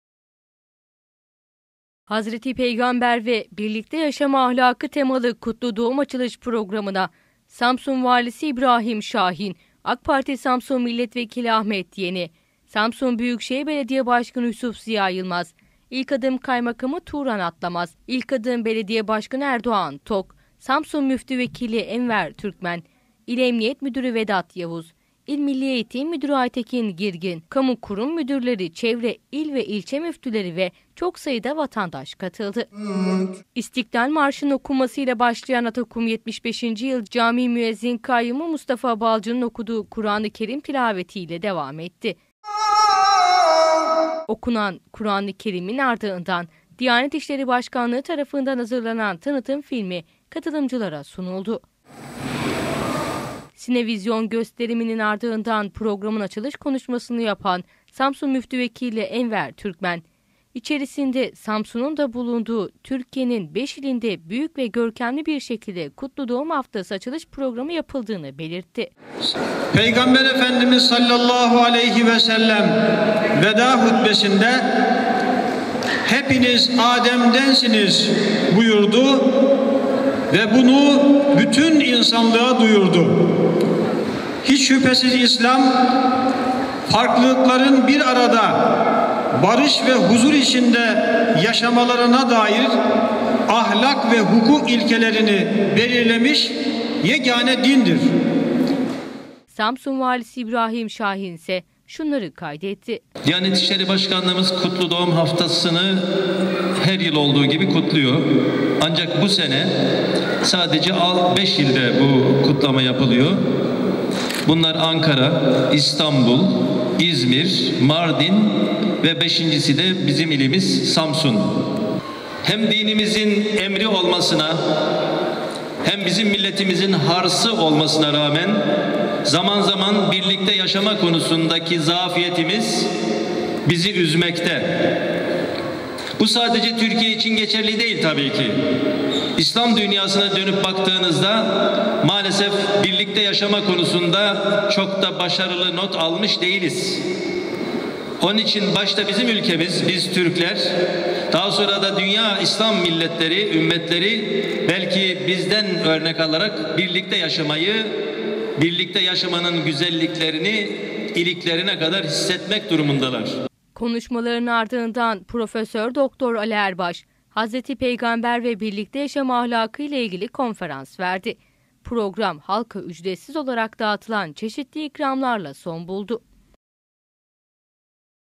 İLKADIM’DA KUTLU DOĞUM PROGRAMINA YOĞUN İLGİ
Muhammed'in (S.A.V) Dünyayı şereflendirdiği 1444. doğum yıl dönümü dolayısıyla Mustafa Dağıstanlı Kapalı Spor  Salonunda program gerçekleştirildi.